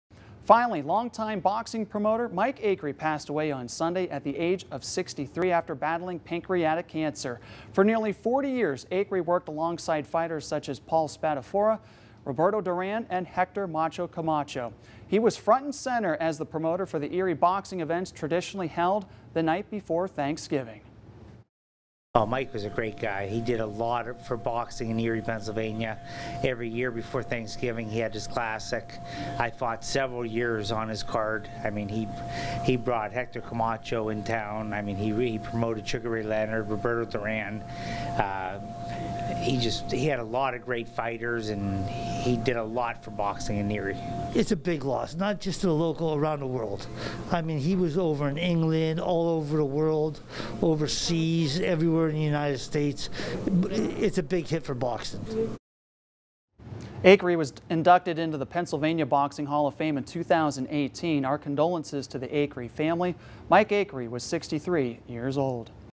Mike Acri Obituary
Louie and John Bizzarro reflect on the passing of boxing promoter Mike Acri